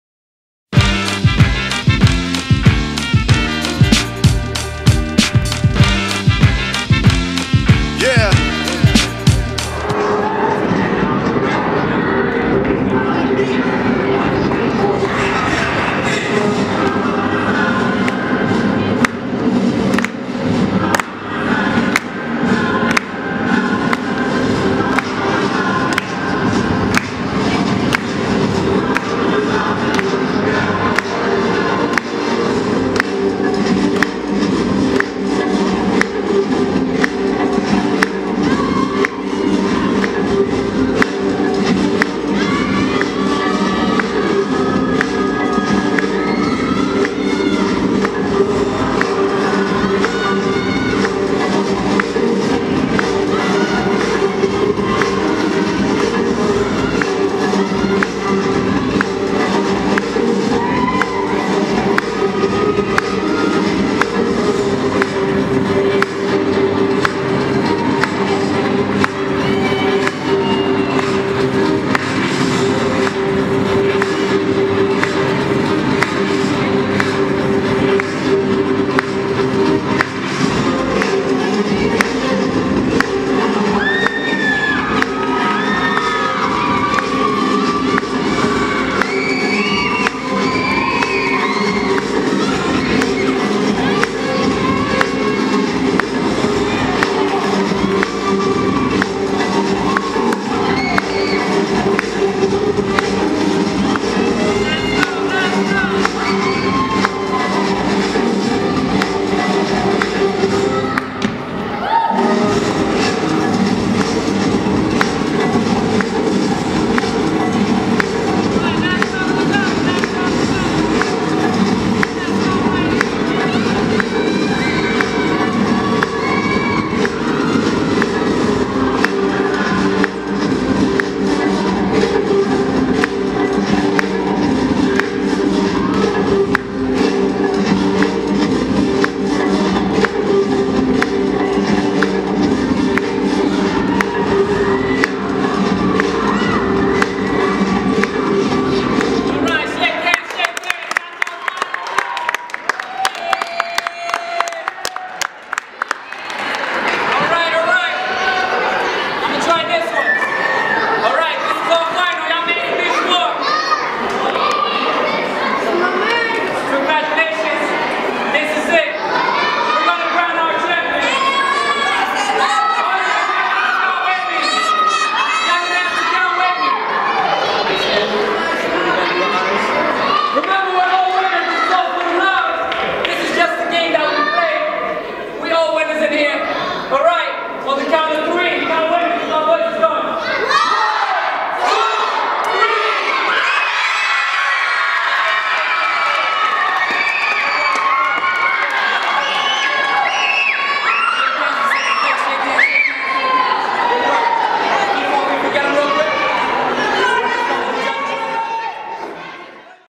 Keith & Kaylin VS X & Ninja . FINAL BATTLE . EPBC Feb 2014
X & Ninja VS Keith & Kaylin
EL Puente Breaking Championship

12 years old & under Final Battle
Winner: X & Ninja